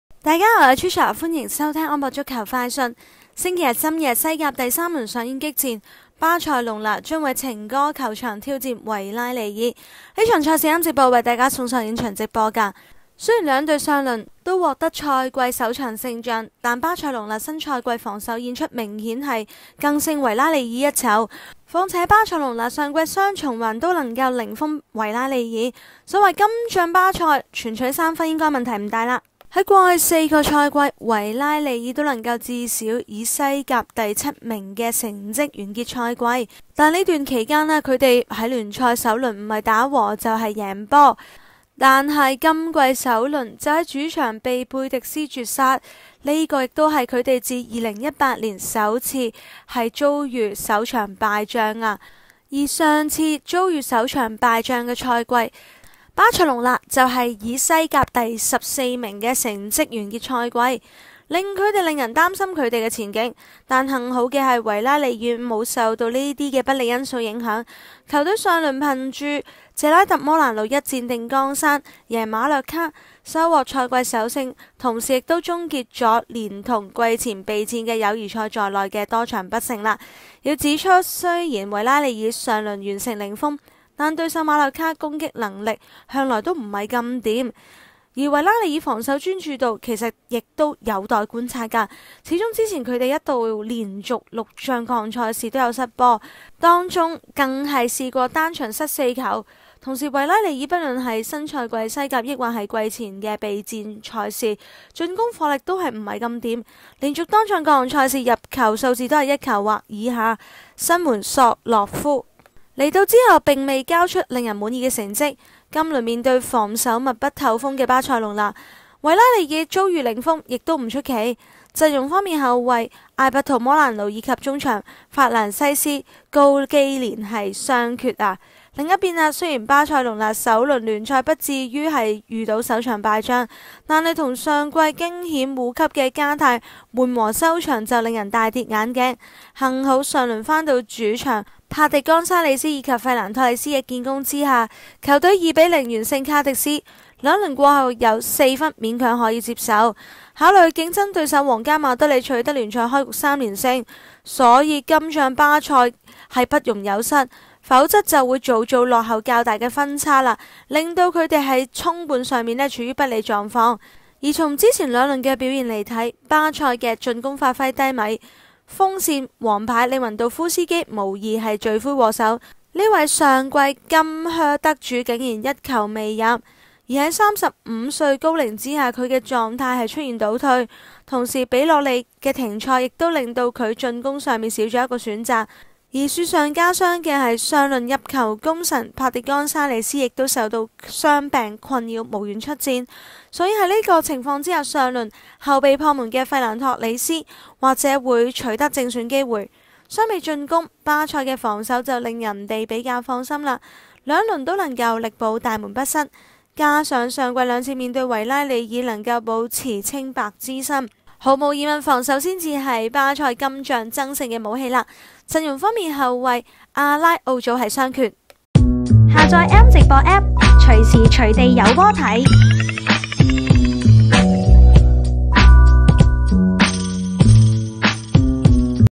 【西甲-賽前分析】2023-08-27 維拉利爾 VS 巴塞隆拿 | 巴塞揮軍有「利」可圖 [聲音報導: Trisha]
週日深夜，西甲 第三輪上演激戰，巴塞隆拿 將造訪情歌球場挑戰#維拉利爾 ，M直播將會有本場賽事的現場直播。雖然兩隊上輪都迎來賽季首勝，但巴塞隆拿新賽季防守演出明顯更勝維拉利爾一籌，況且紅藍軍團上季雙循環均零封維拉利爾，巴塞隆拿此行全取3分問題不大。

過去4個賽季，維拉利爾都至少能夠以西甲第七名收官，在此期間，黃色潛艇聯賽首輪非和即勝。然而，今季首輪球隊卻在主場遭到貝迪斯絕殺，這是他們自2018年後首次遭遇開門黑，而上次遭遇開門黑的賽季，球隊僅以西甲第十四名收官，不禁令人擔心他們今季前景。所幸的是維拉利爾沒有受到這些不利因素所影響，球隊上輪憑藉謝拉特.摩蘭奴的一箭定江山力挫馬略卡，收獲賽季首勝的同時也終結了連同季前備戰的友誼賽在內多場不勝。需要指出的是，雖然黃色潛艇上輪完成零封，但對手馬略卡進攻能力向來不濟，前者防守專注度仍有待觀察，畢竟在此之前他們一度連續6仗各項賽事失球，不乏單場失4球的災難演出；與此同時，維拉利爾不論是新賽季西甲抑或是季前備戰時進攻火力均十分平庸，連續多仗各項賽事入球數在1球或以下，新援阿歷山大.索洛夫到來後並未交出令人滿意答卷，本輪面對防守密不透風的巴塞隆拿，維拉利爾遭遇零封不足為奇。陣容方面，後衛艾拔圖.摩蘭奴和中場法蘭西斯.高基連傷缺。

另一邊廂，雖然巴塞隆拿首輪聯賽不至於遭遇開門黑，但與上季驚險保組的加泰悶和收場顯然讓人大跌眼鏡，幸好上輪回到主場在柏迪.干沙里斯和費蘭.托里斯的建功下，球隊2比0完勝卡迪斯，兩輪過後積4分勉強可以接受。考慮到競爭對手皇家馬德里取得聯賽開局三連勝，因此紅藍軍團本輪出征不容有失，否則早早便落後較大分差對球隊之後衝冠十分不利。從前兩輪表現來看，巴塞隆拿進攻發揮低迷，鋒線王牌羅拔.利雲度夫斯基無疑是罪魁禍首，這位上季西甲金靴得主一球未入，35歲高齡的他狀態出現了倒退，同時拉菲拿.比洛利的停賽也讓球隊進攻端少了一個選擇，雪上加霜的是上輪入球功臣之一柏迪.干沙里斯也受到傷病困擾無緣此戰，在此情況下，上輪後備破門的費蘭.托里斯或將獲得正選機會。相比進攻，巴塞隆拿的防守則令人十分放心，兩輪均能力保大門不失，加之上季2次面對維拉利爾亦能保持清白之身，毫無疑問在，防守將是紅藍軍團此行的爭勝武器。陣容方面，後衛朗奴.阿拉奧祖傷缺。